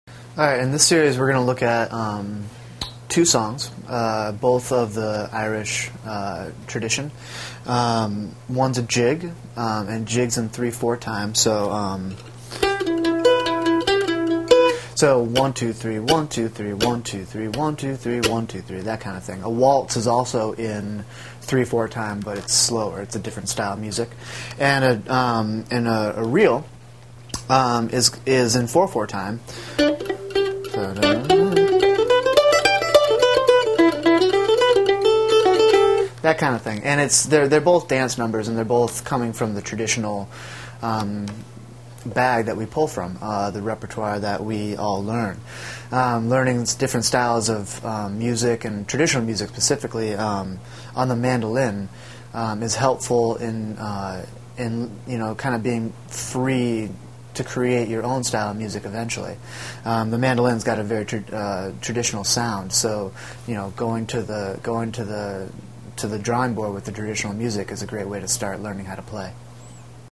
Irish Jig Mandolin Tips
Irish Jig Mandolin Tips. Part of the series: Irish Jig Mandolin Music Lessons. How can you do a happy jig without knowing what a jig actually is? Learn about Irish Jigs and how to play them on the mandolin from a musician in this free video clip.